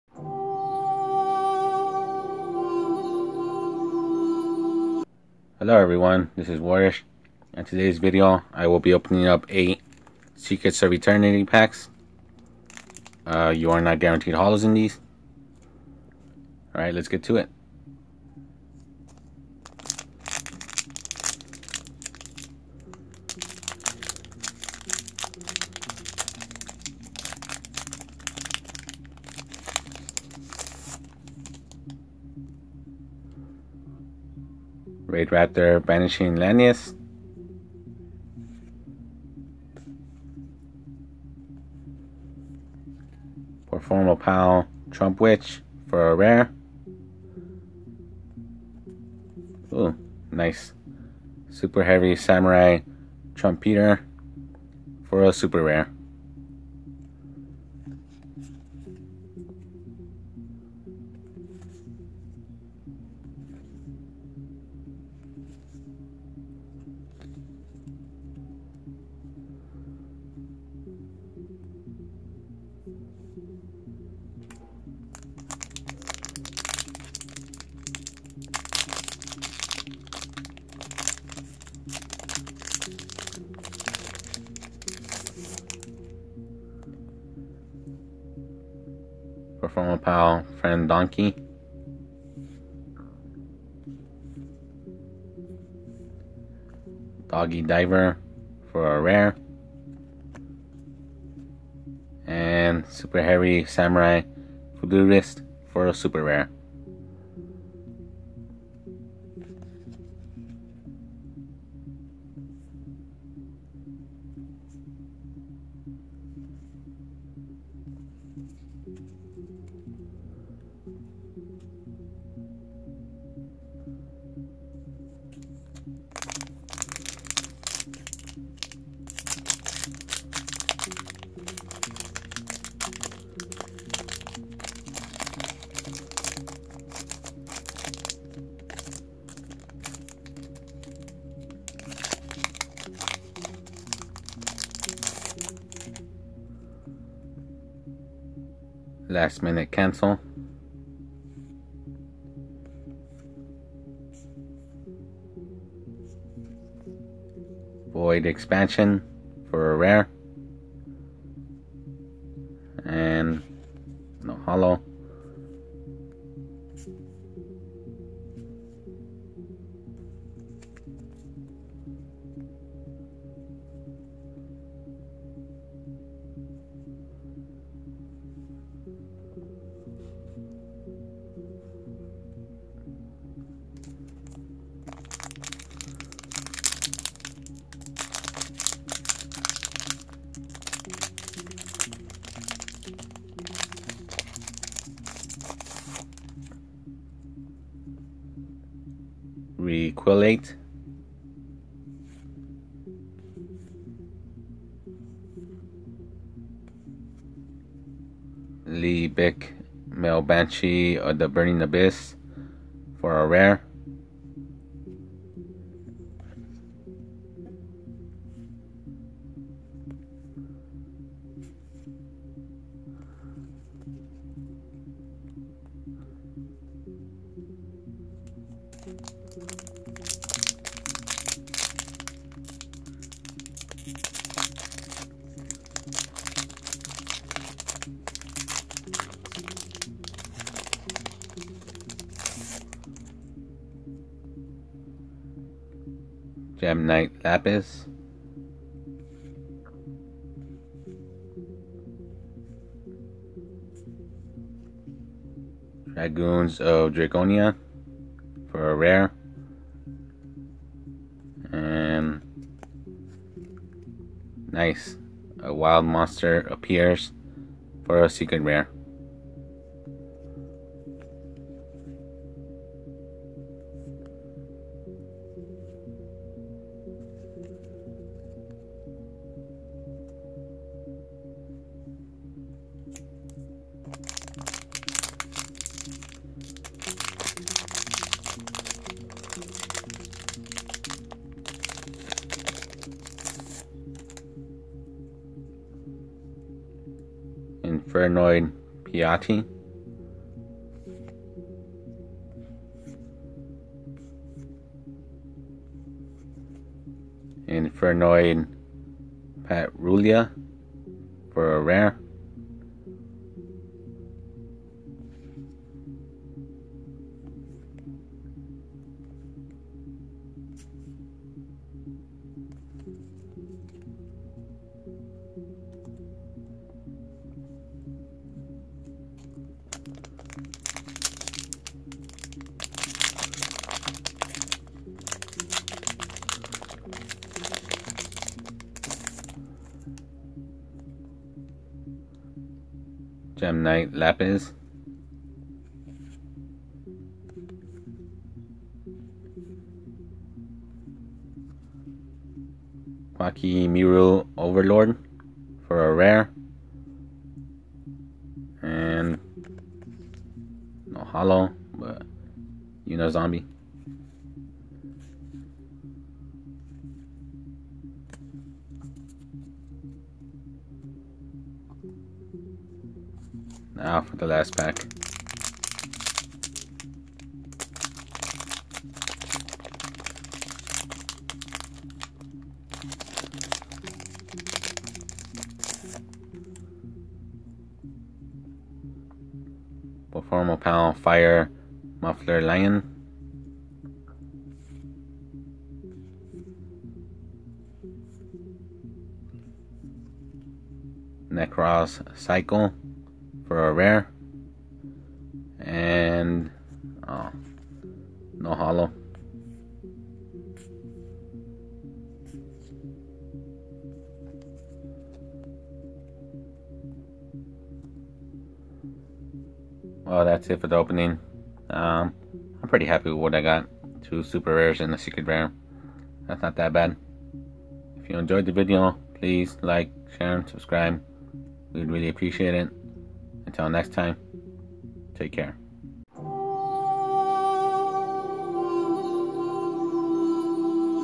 Yu-Gi-Oh! Secrets of Eternity pack x8 opening.
My Yu-Gi-Oh! Secrets of Eternity pack opening. What other openings would you want to see?